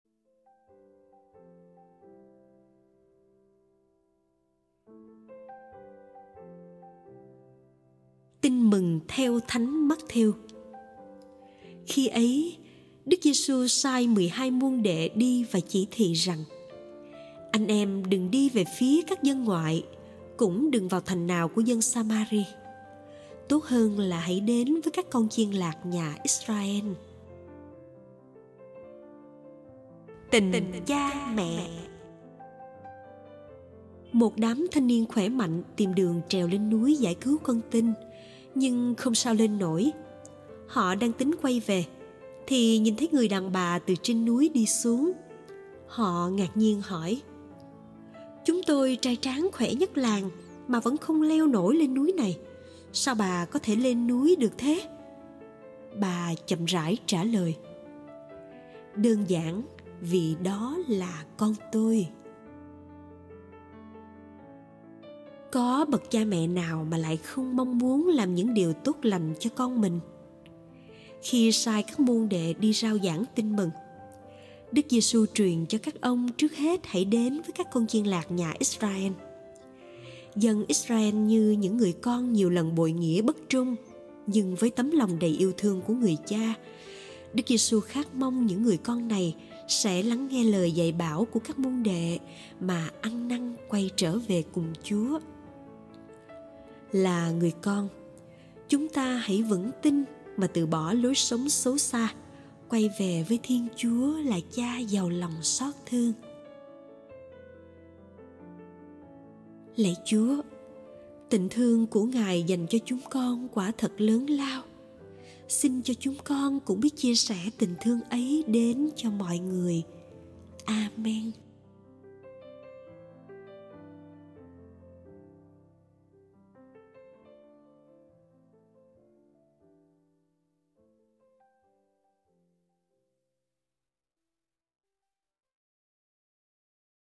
Chấm Nối Chấm 2015: 8.7: Tình Cha Mẹ
THỨ TƯ 08.07 - Tuần XIV Thường Niên
St 41:55-57; 42:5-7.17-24a; Mt 10:1-7

Khi ấy, Đức Giê-su sai mười hai môn đệ đi và chỉ thị rằng: “Anh em đừng đi về phía các dân ngoại, cũng đừng vào thành nào của dân Sa-ma-ri. Tốt hơn là hãy đến với các con chiên lạc nhà Ít-ra-en.”

TÌNH CHA MẸ

Một đám thanh niên khỏe mạnh tìm đường trèo lên núi giải cứu con tin nhưng không sao lên nổi. Họ đang tính quay về thì nhìn thấy người đàn bà từ trên núi đi xuống. Họ ngạc nhiên hỏi: “Chúng tôi trai tráng khỏe nhất làng mà vẫn không leo nổi lên núi này. Sao bà có thể lên núi được thế?” Bà chậm rãi trả lời: “Đơn giản vì đó là con tôi”.

Có bậc cha mẹ nào mà lại không mong muốn làm những điều tốt lành cho con mình?

Khi sai các môn đệ đi rao giảng Tin Mừng, Đức Giêsu truyền cho các ông trước hết hãy đến với các con chiên lạc nhà Israel. Dân Israel như những người con nhiều lần bội nghĩa bất trung, nhưng với tấm lòng đầy yêu thương của người cha, Đức Giêsu khát mong những người con này sẽ lắng nghe lời dạy bảo các môn đệ mà ăn năn quay trở về cùng Chúa.

Là người con, chúng ta hãy vững tin mà từ bỏ lối sống xấu xa, quay về với Thiên Chúa là Cha giàu lòng xót thương.

Lạy Chúa, tình thương của Ngài dành cho chúng con quả thật lớn lao. Xin cho chúng con cũng biết chia sẻ tình thương ấy đến cho mọi người. A-men.

Nội dung: Học viện Đa-minh
Slides: Truyền Thông Đa-minh